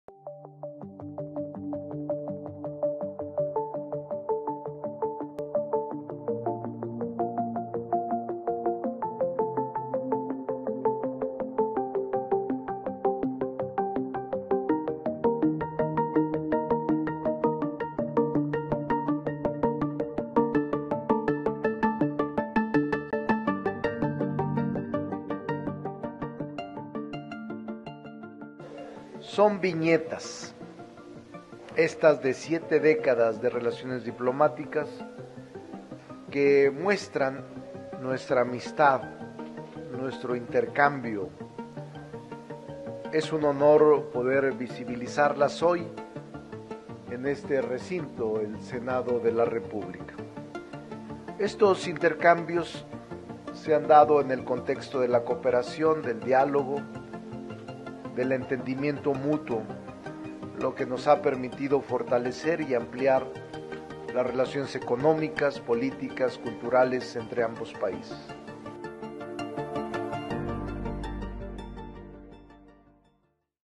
¿Sabían que la relación México-Israel cumplió más de 70 años?
¿Sabían que la relación México-Israel cumplió más de 70 años? Para conmemorar la diplomacia entre ambas naciones, el presidente de la Junta de Coordinación Política, Ricardo Monreal, inauguró una exposición.